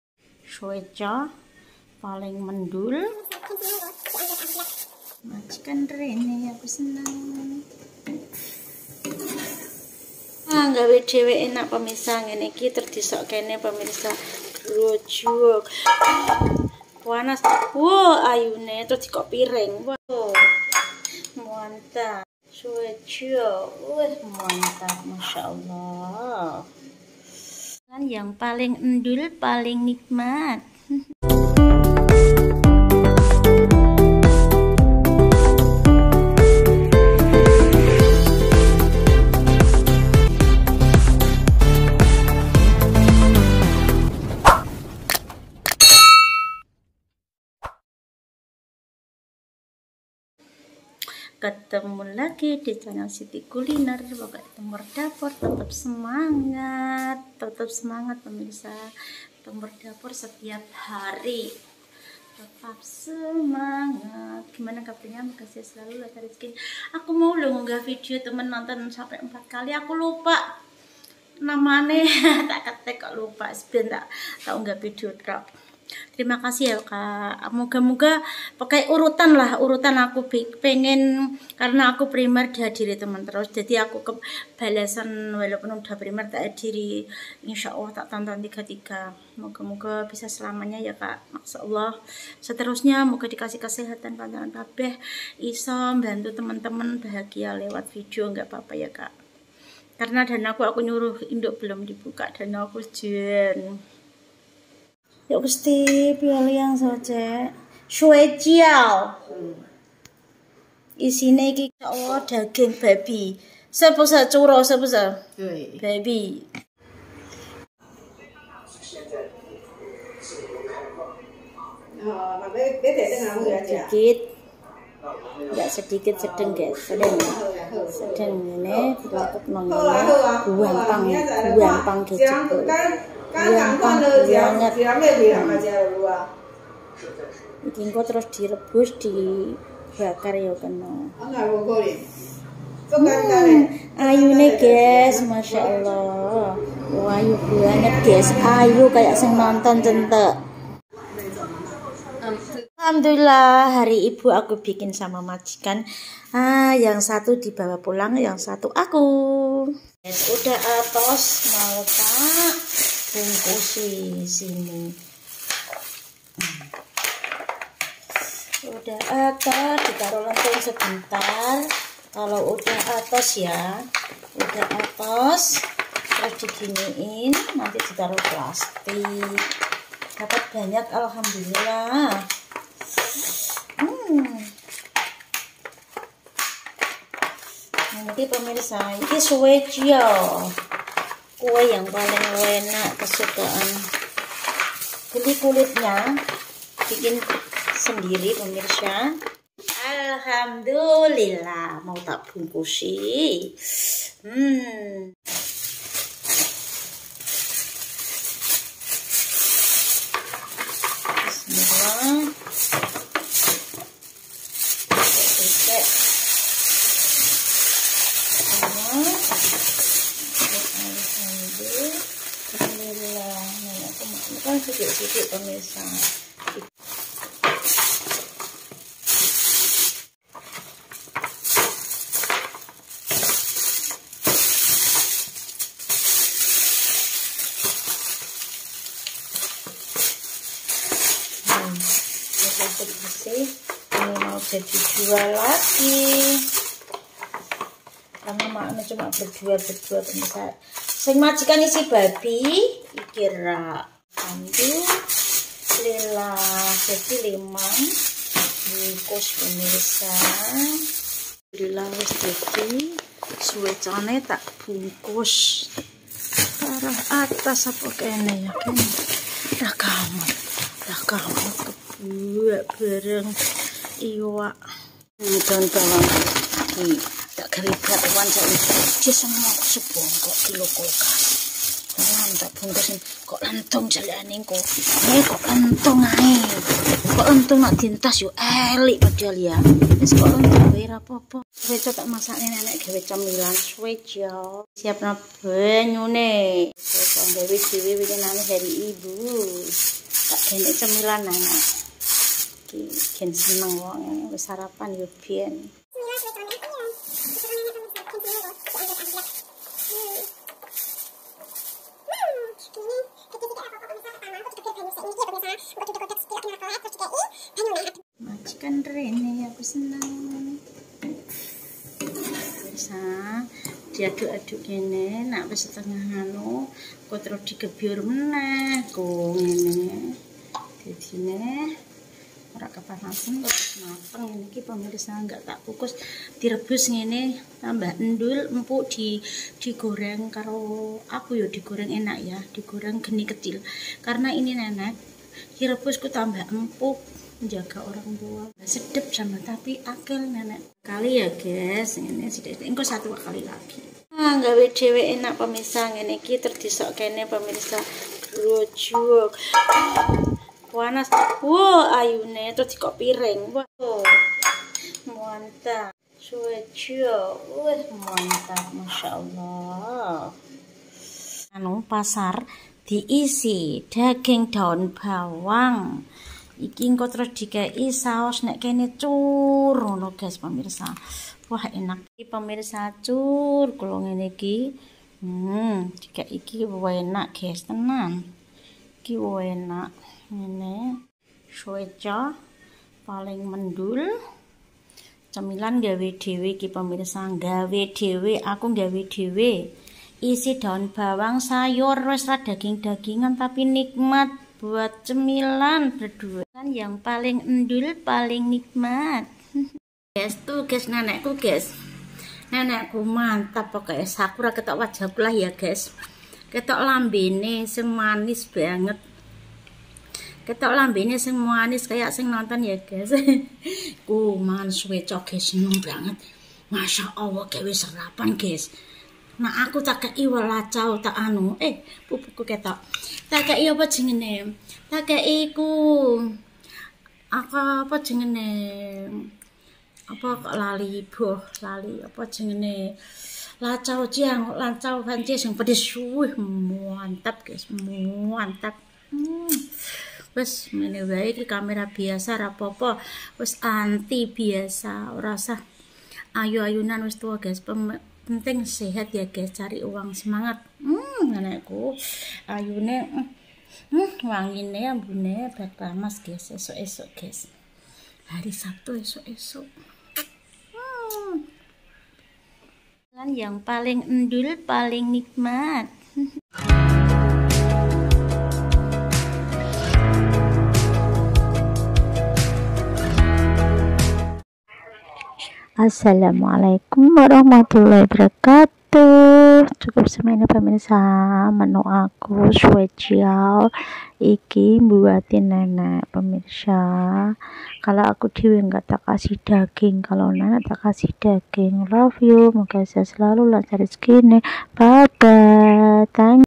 Resep & Cara Membuat Dumpling ll Sueciau Chinese Food & Mudah Dan Enak
Resep & cara membuat Dumpling ll Sueciau Chinese food Mudah & Enak @sitykuliner2697
Resep Cara Membuat Dumpling atau juga bisa di sebut sue ciau  Chinese food maupun di sini saya membagi resep yang sangat mudah dan enak , buat kalian yang belum bisah. membuat dumpling bisa lihat video lengkap TKW Taiwan membuat dumpling 😄
untuk Dumpling ini kalian bisa menggunakan berbagai daging ayam, daging sapi daging babi, maupun hanya sayuran saja,
untuk pembuatan Dumpling bias di sesuaikan selera kalian masing-masing yah
semoga kalian suka dan puas atas resep
dan cara sederhana yang saya bagikan
Trimakasih wasalam mualaikum wr wb 😘